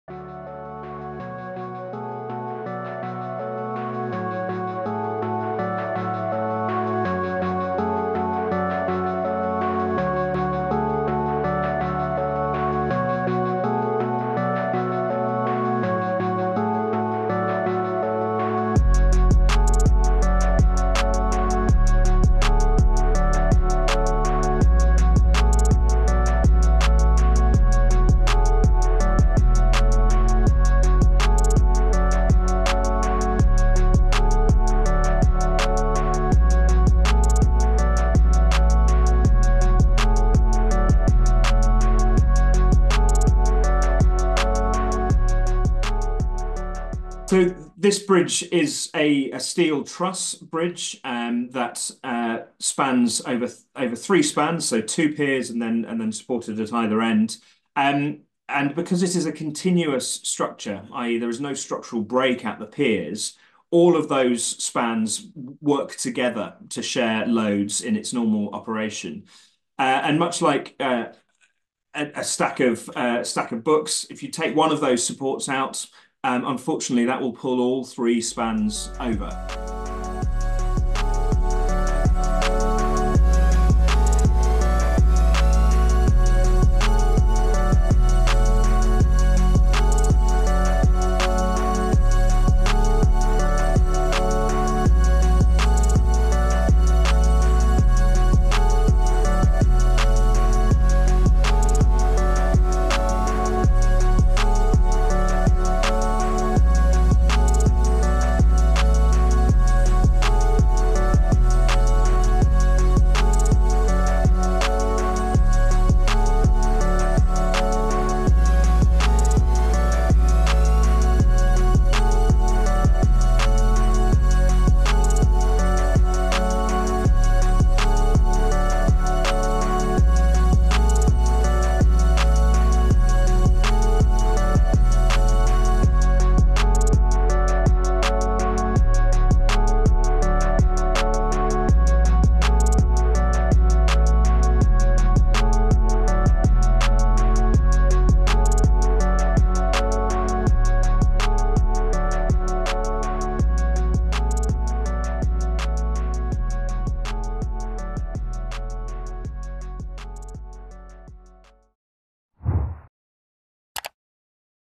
巴爾的摩大橋遭貨輪撞 專家分析急速崩塌原因 衛星照看意外前後對比｜Yahoo Hong Kong
美國馬里蘭州巴爾的摩市凱伊橋（Francis Scott Key bridge）在當地時間3月26日凌晨，遭貨輪撞上後應聲斷裂倒塌，空拍畫面顯示，貨輪船頭與破碎的橋梁交錯；衛星對比圖也清楚呈現橋梁被撞前後狀況。至於橋梁何以崩塌如此迅速？英國結構土木工程師奈特（David Knight）解釋，凱伊橋是鋼製桁架橋（truss bridge），有3個跨度（span），屬於連續結構且兩端沒有支撐，如果拿掉其中一個支撐物（橋墩）就會倒塌。 奈特表示，這種橋梁雖然帶來視野廣闊、用料減少、容易建造的好處，但像這種「極端事件」發生時，略顯缺乏穩定性，而現代經過專門設計，這種橋梁應可承受此類碰撞，且在水中都會為橋梁加固。他表示，桁架橋在美國十分常見，特別是海岸周圍的公路網，總體上來說是有彈性且安全的結構。 截至台灣時間27日早上，海岸警衛隊稱6名失蹤工人恐已遇難，搜救行動也宣布告終。事故原因尚待釐清，最新消息指，這艘懸掛新加坡國旗、長289公尺的達利（DALI）號貨輪，事發前通報當局，船隻完全失去動力，官員急忙通報交通管制。 美國國家運輸安全委員會（NTSB）預計在27日登船，另外還會調查橋梁的結構等，總統拜登表示，聯邦政府將負擔橋梁重建的費用。交通當局表示，這起意外使該航道「無限期關閉」，將嚴重影響有關供應鏈與當地經濟。